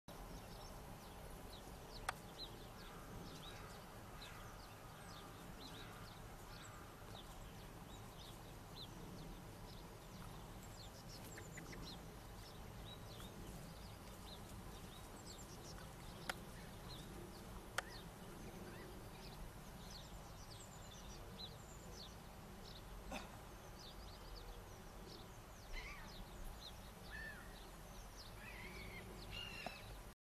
American Robin
I took this footage of an American Robin, a very rare visitor to the UK, in Bingley (West Yorkshire). I managed to get my brother in the video who was taking pictures of the bird on his camera!